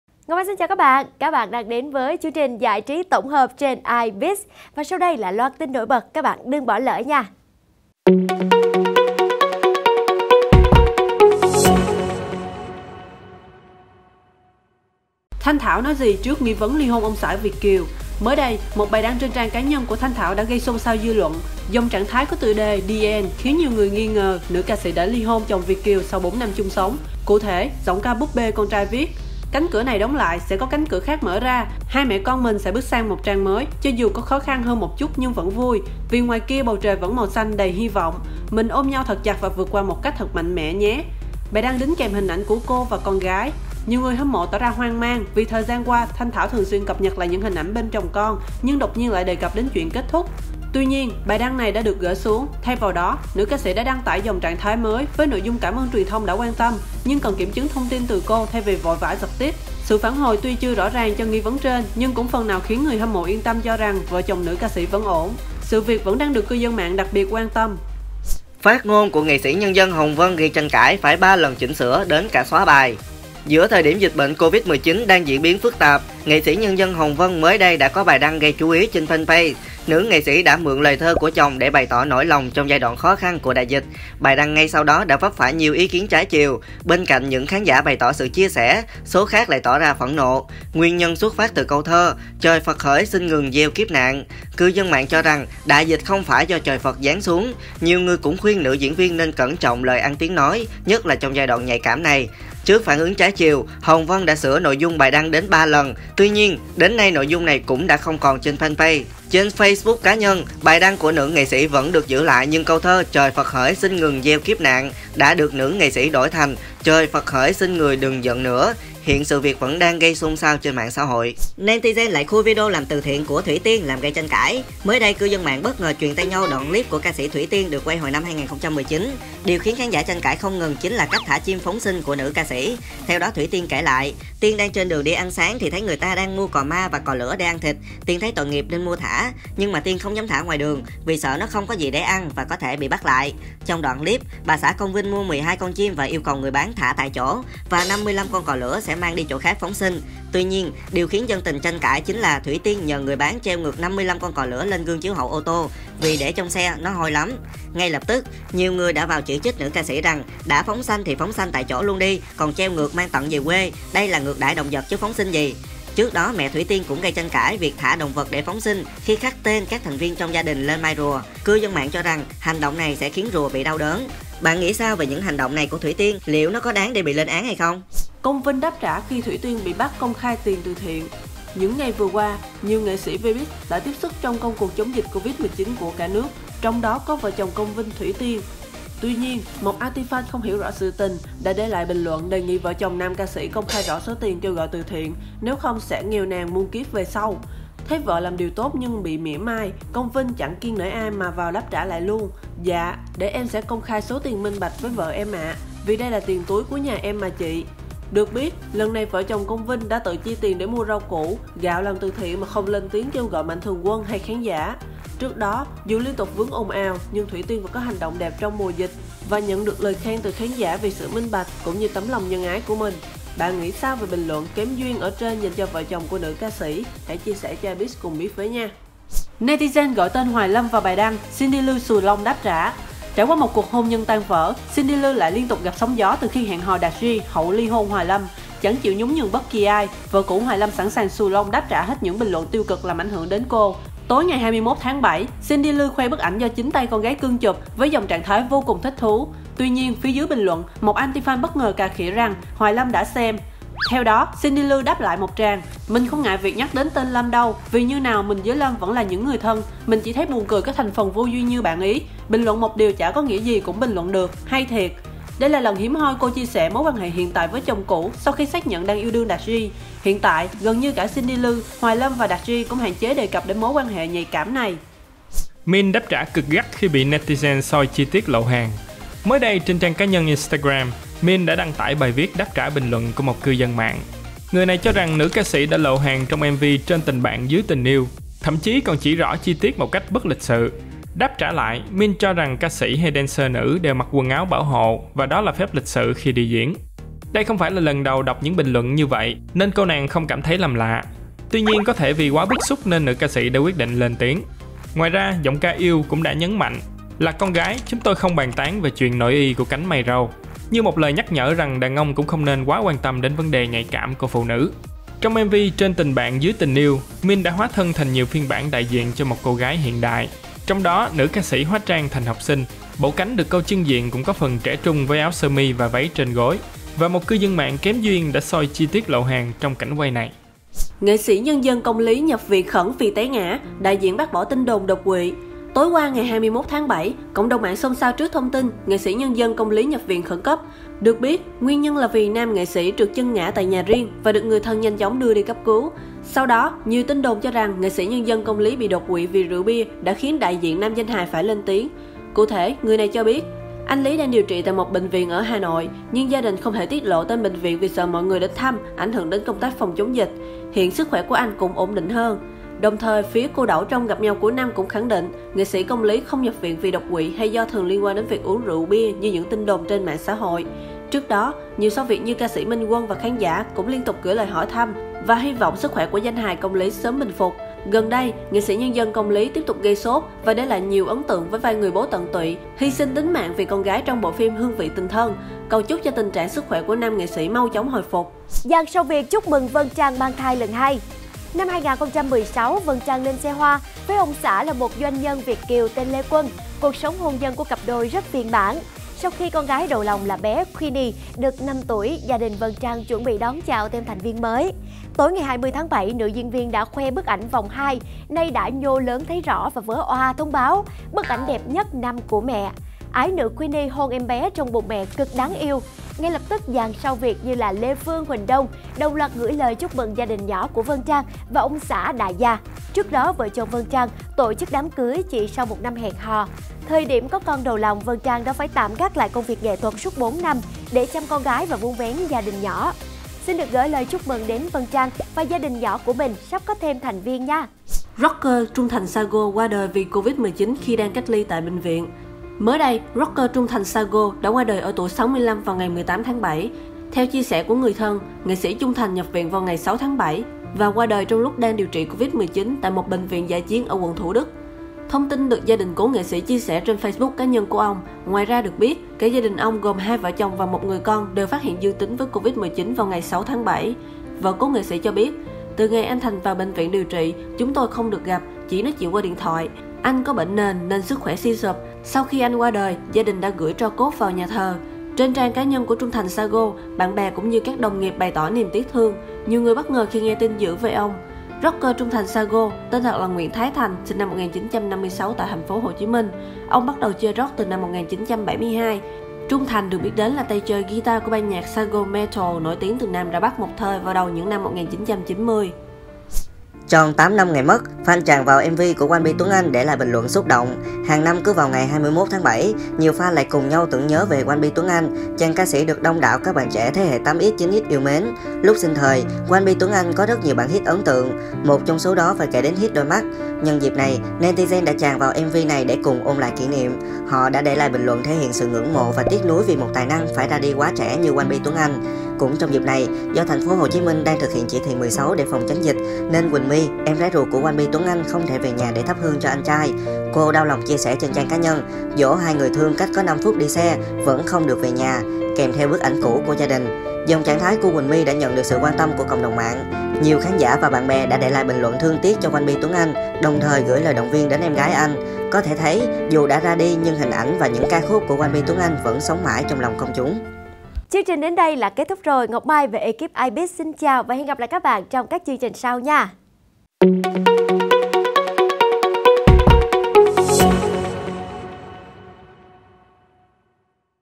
Thanh Thảo ly hôn chồng Việt kiều? | NSND Hồng Vân, Thủy Tiên lại gây tranh cãi gay gắt | Ibiz
Thanh Thảo nói gì trước nghi vấn ly hôn ông xã Việt kiều
Phát ngôn của NSND Hồng Vân gây tranh cãi phải 3 lần chỉnh sửa đến cả xóa bài
Netizen lại khui video làm từ thiện của Thuỷ Tiên làm gây tranh cãi
Công Vinh đáp trả khi Thuỷ Tiên bị bắt công khai tiền từ thiện
Netizen gọi tên ‘Hoài Lâm’ vào bài đăng, Cindy Lư ‘xù lông’ đáp trả
Min đáp trả cực gắt khi bị netizen soi chi tiết “lộ hàng”
NSND Công Lý nhập viện khẩn vì đột quỵ?
Dàn sao Việt chúc mừng vân trang mang thai lần 2
Nam nghệ sĩ Việt qua đời tại bệnh viện vì covid-19
Tròn 8 năm ngày mất, fans tràn vào MV của Wanbi Tuấn Anh để lại bình luận xúc động